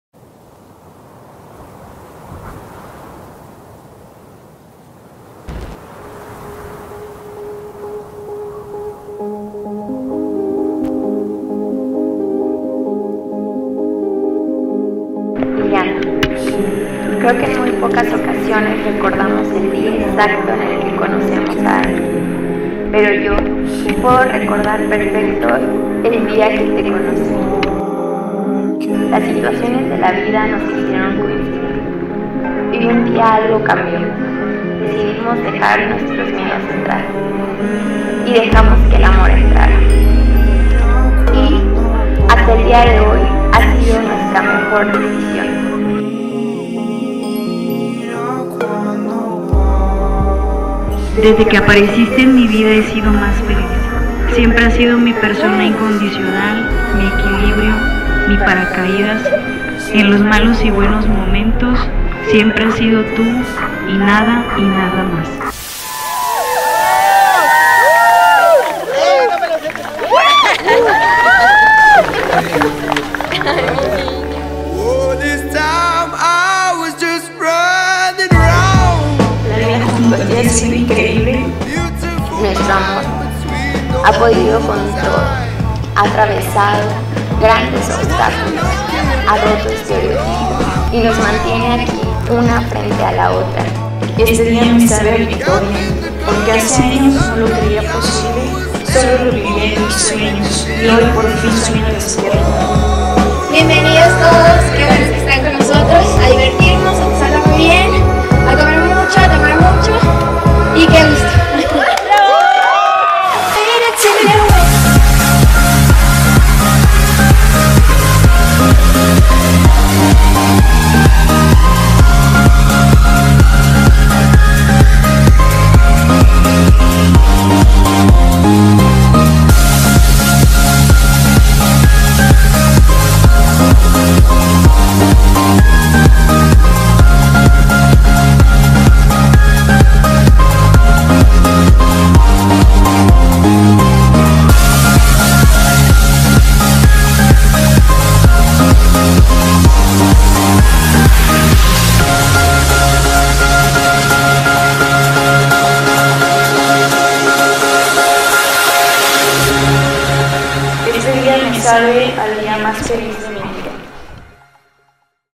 GABY + ILIANA (SHORT FILM)
Wedding Day filmed in Ex-Hacienda de Chautla, Puebla-Mex. by Rockstar Wedding.